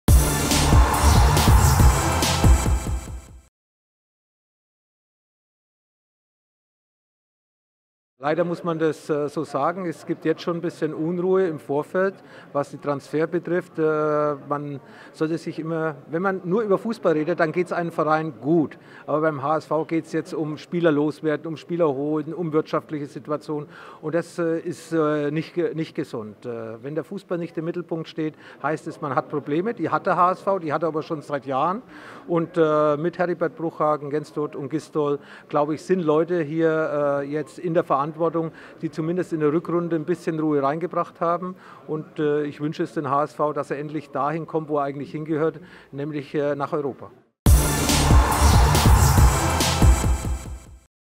"Der HSV gehört nach Europa!" - Interview mit Lothar Matthäus
Der Rekordnationalspieler im Gespräch

"Der HSV hat Probleme" - Experte Lothar Matthäus kommentiert die Situation der Rothosen. Ob es auch in diesem gegen den Abstieg geht?